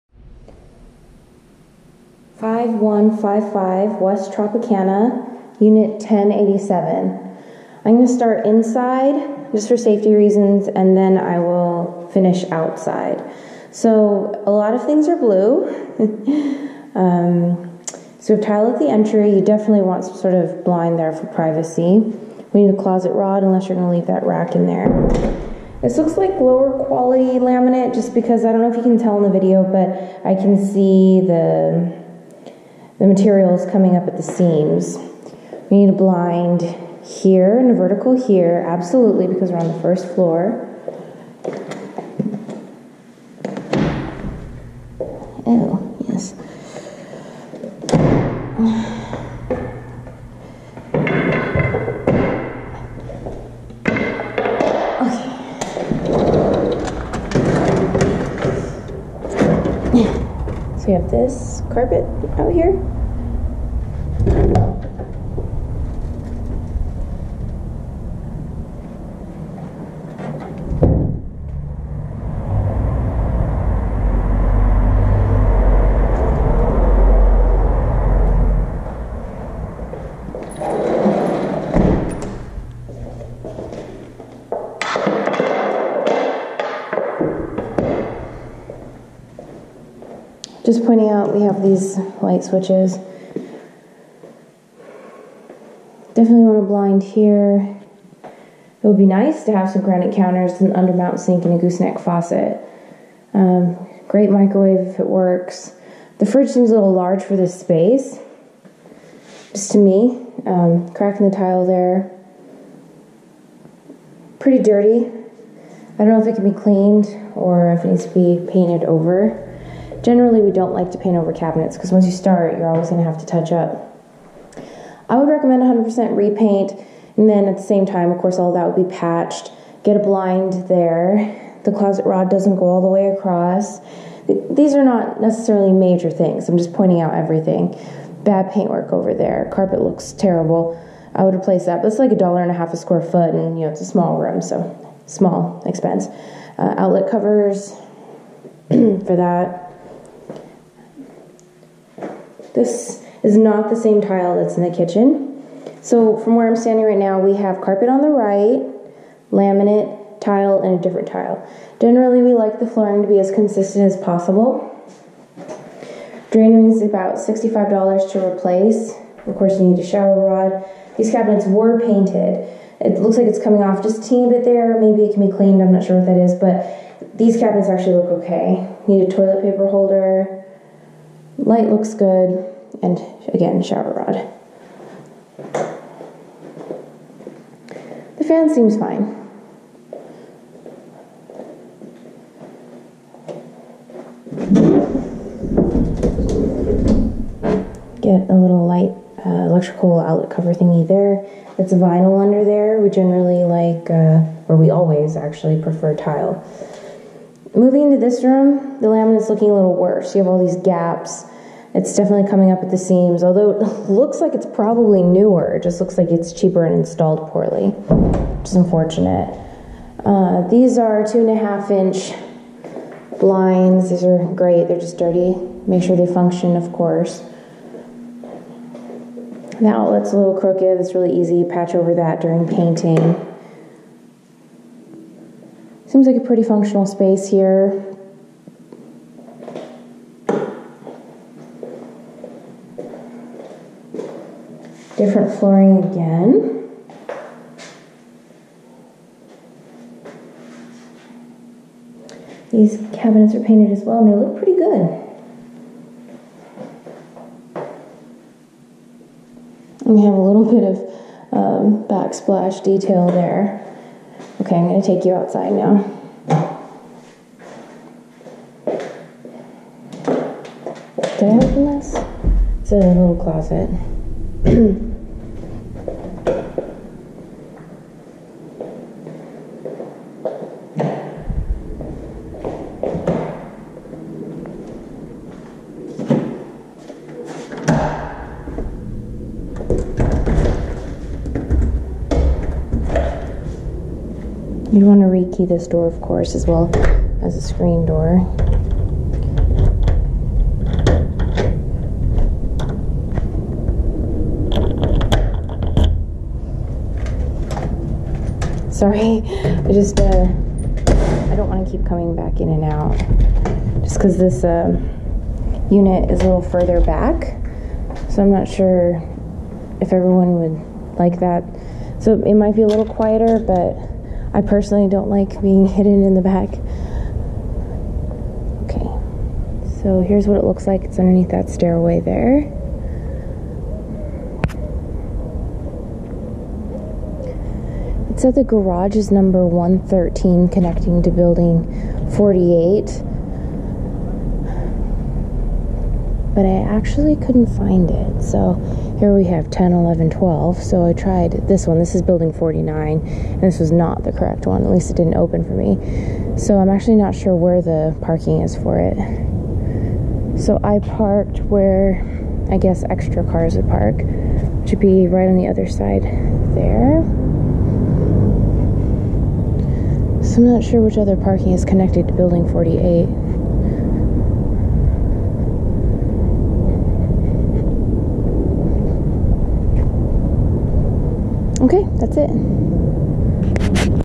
2125266 5155 W Tropicana ave 1087
Another property evaluation by Las Vegas Real Estate Investment Group. This is a floor level unit with a parking garage.